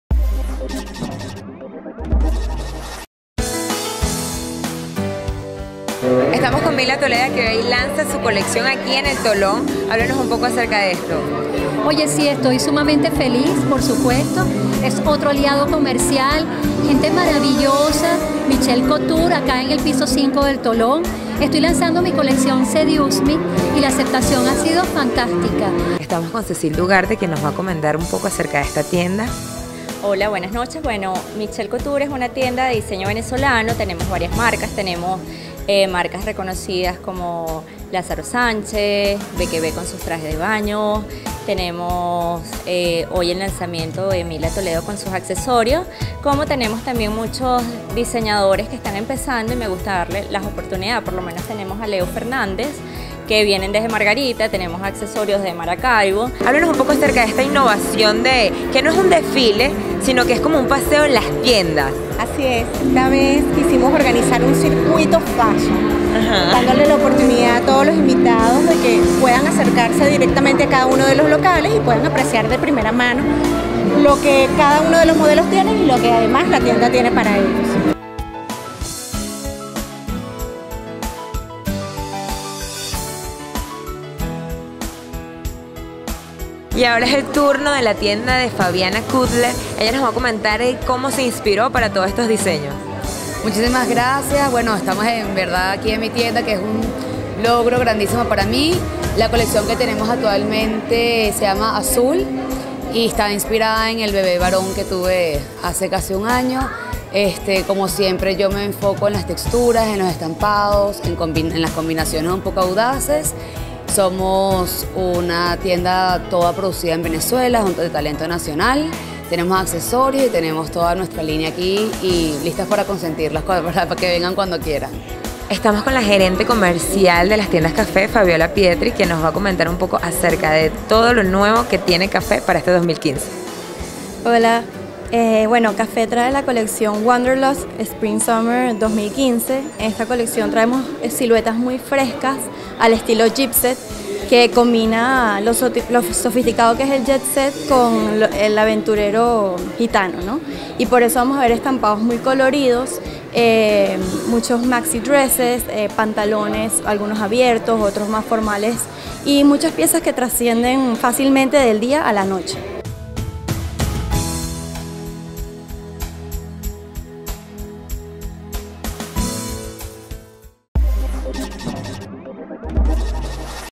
La primavera-verano llegó a piso 5 de Tolón
En una original presentación las bellas modelos instaladas a las puertas de cada tienda, mostraron con mucha gracia como interpretan la primavera y el verano 2015, los diseñadores de las casas de la talla y prestigio de Talento, Michelle Couture, Fabiana Kubler, Bindi, Villebrequin, Caffe Swimwear, Mansion Dantes y Adolfo Domínguez.